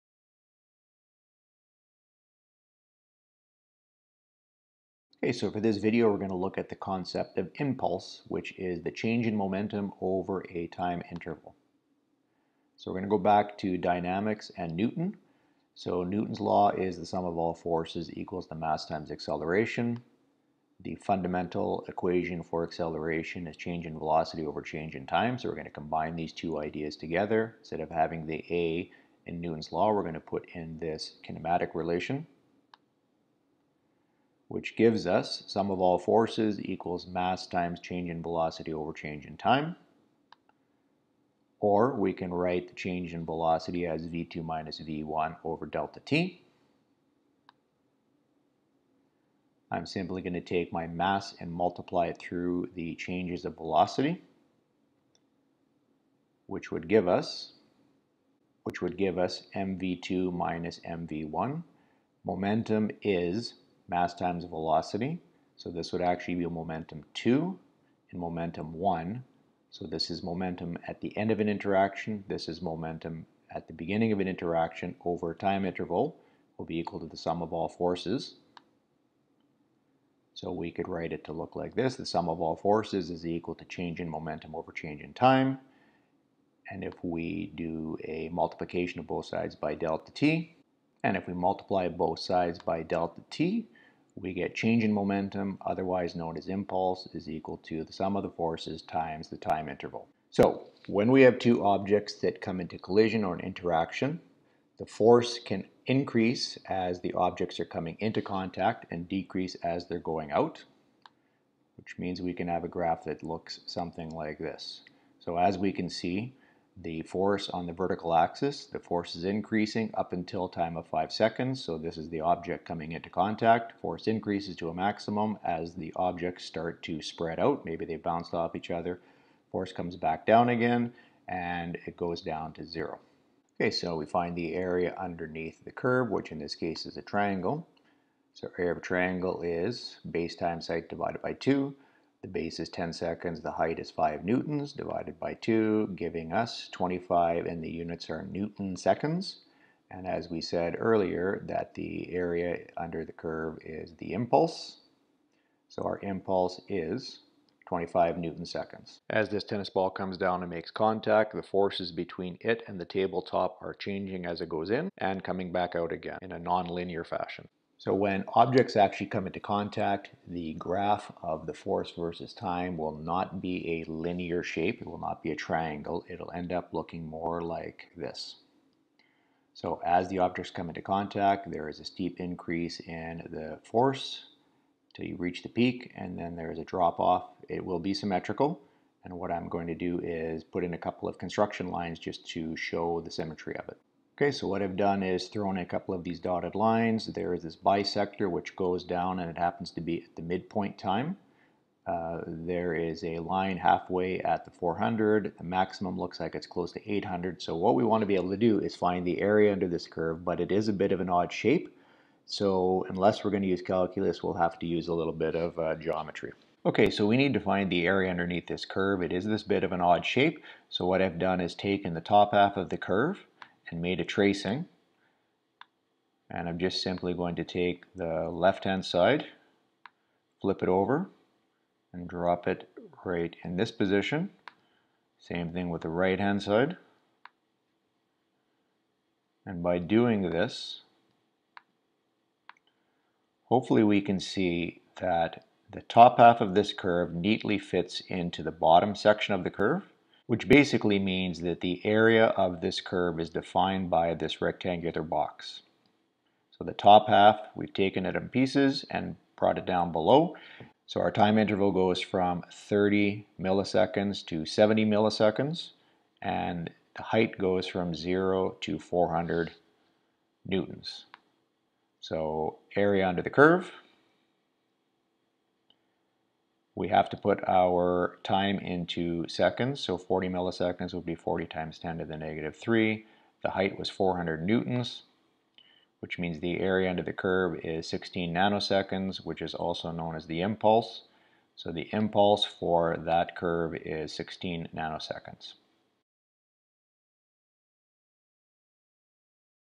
Momentum & Impulse & Impulse from a F vs. t graphs
A derivation of Impulse from Newtons Laws and Kinematics, and how impulse is found from a F vs. t graph (without using Calculus)